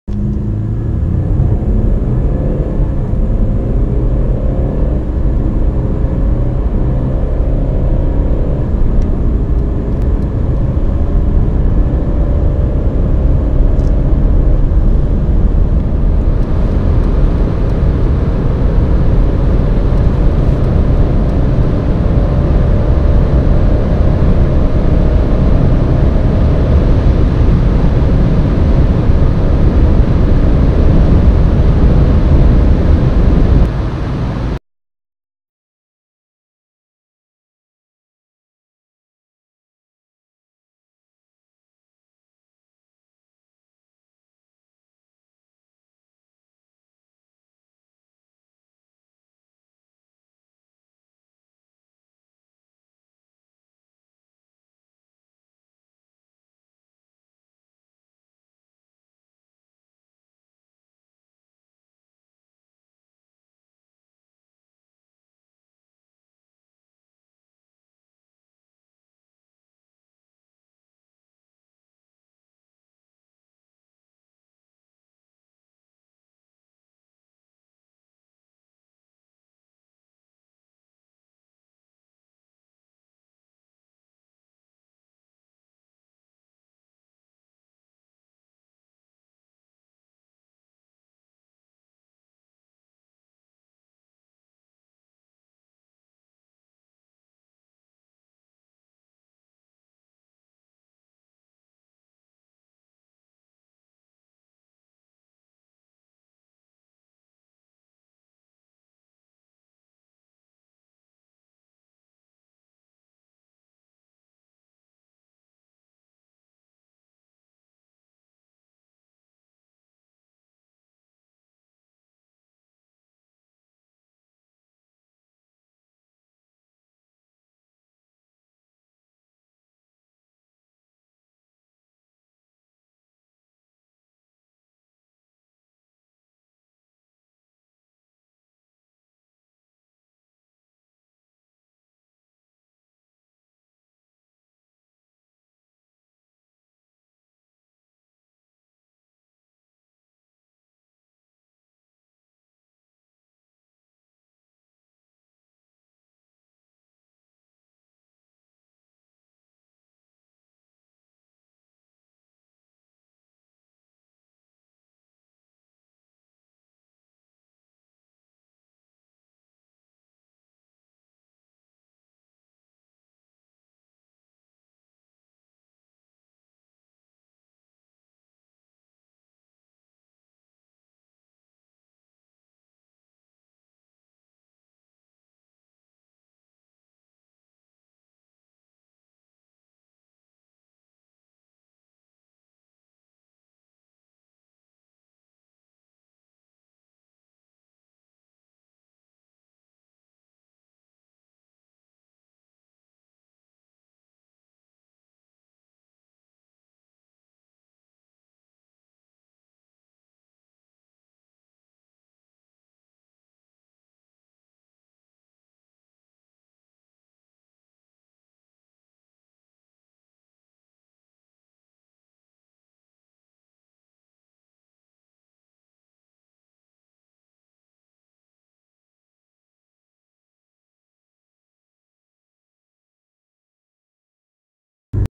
2021 G20 BMW 320D XDRIVE 190PS VS W212 MERCEDES E220D 194PS ACCELERATION 0-200KM/H SUPRISE RESULT ??
2021 G20 BMW 320D XDRIVE 190PS VS W212 MERCEDES E220D 194PS ACCELERATION 0-200KM/H SUPRISE RESULT ??
BMW 320D XDRIVE
2L I4 TURBO 190/400NM
VS
MERCEDES E220D W212
2L I4 TURBO 194/400
ACCELERATION
0-200KM/H